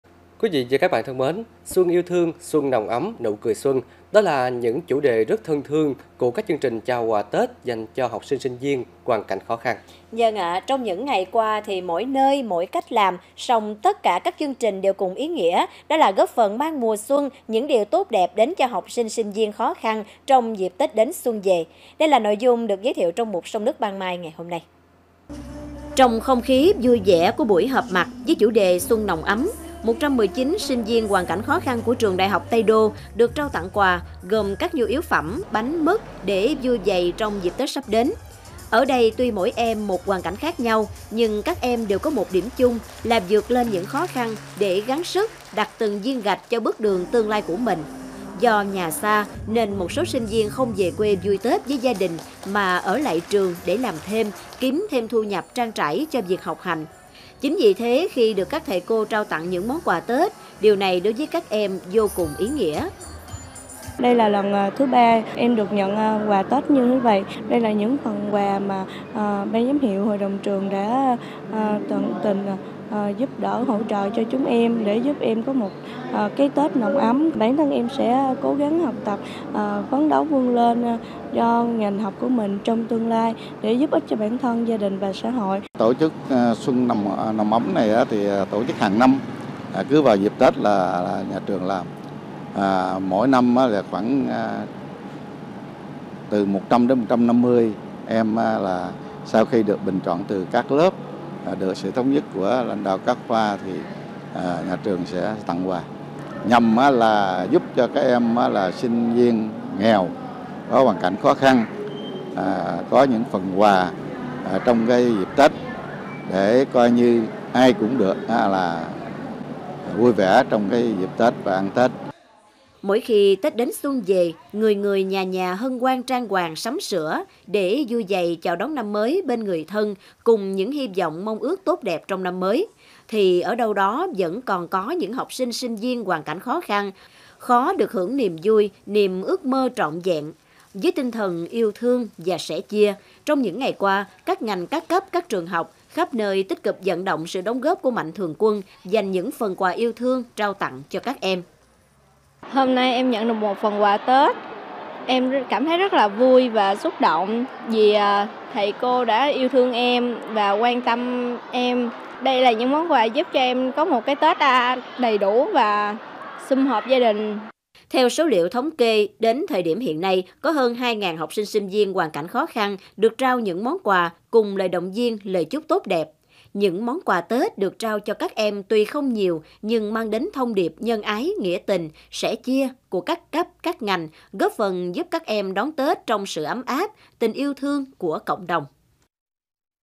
Mùa xuân nồng ấm yêu thương| Cần Thơ TV
“Xuân yêu thương”, “Xuân nồng ấm”, “Nụ cười Xuân”… đó là những chủ đề rất thân thương của các chương trình trao quà Tết dành cho học sinh sinh viên hoàn cảnh khó khăn.